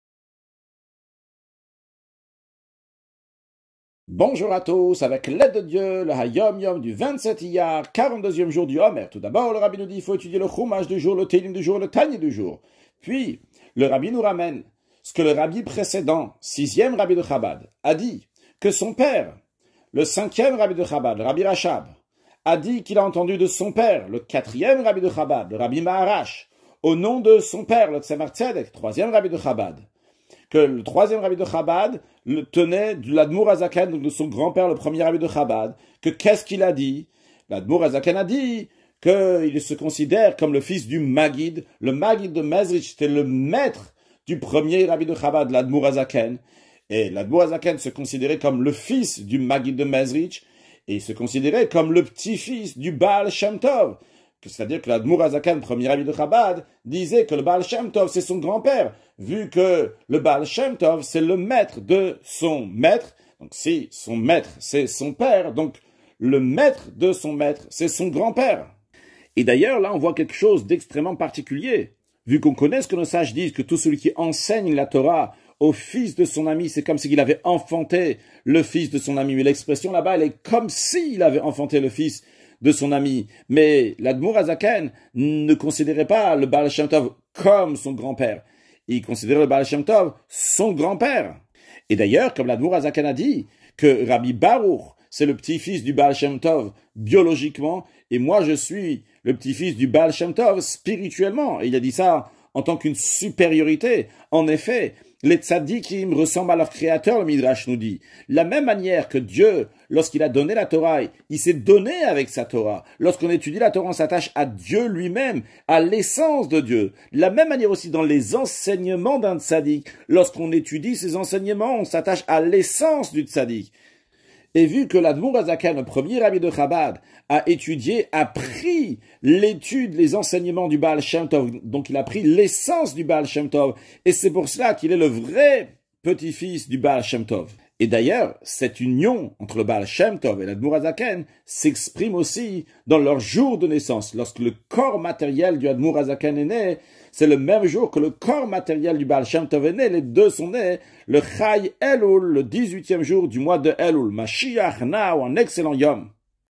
Le petit fils du Baal Chem Tov !
Hayom yom 27 iyar - 42e jour de l'Omer
Passages à étudier : ‘Houmach : Chélichi de Bamidbar avec Rachi
Tehilim : 120 à 134
Tanya : Vekol Ka’h Atsmou... Eïn Sof Barou’h Hou

Mon père (le Rabbi Rachab) rapporta ce qu’il entendit de son père, au
nom du Tséma’h Tsédek, qui lui-même le tenait de l’Admour Hazaken: ce
dernier se considérait comme le fils du Maguid (son maître) et le petitfils
du Baal Chem Tov.